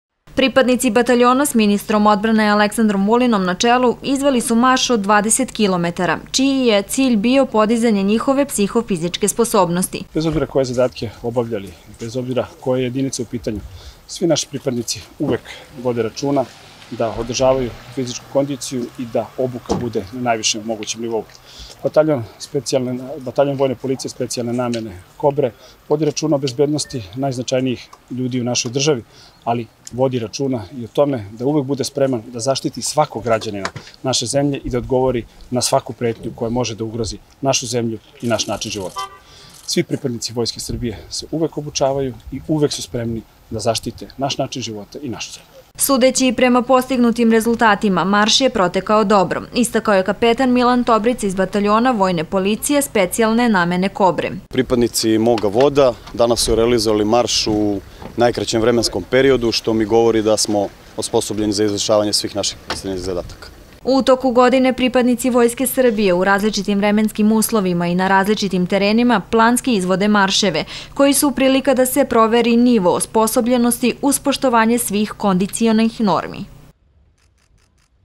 Ministar Vulin u kondicionom maršu sa "Kobrama"
Pripadnici Bataljona vojne policije specijalne namene "Kobre" su u okviru redovnih planskih aktivnosti, izveli kondicioni marš na relaciji Ripanj - Parcani - Ralja, u kojem je učestvovao i ministar odbrane Aleksandar Vulin